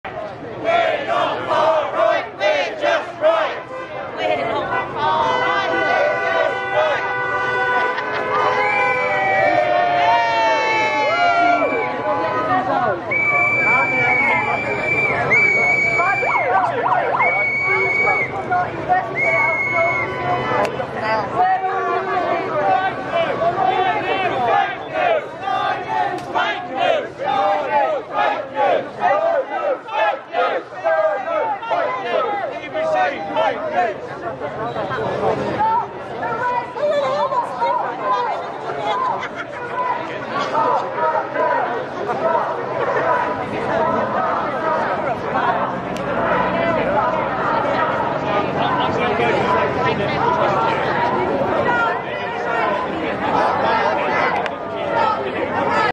UK Yellow Vests protest London. 2nd February 2019.
With chants of “We're not far right, we're just right” hundreds of UK Yellow Vests protest Westminster, London 2nd February 2019.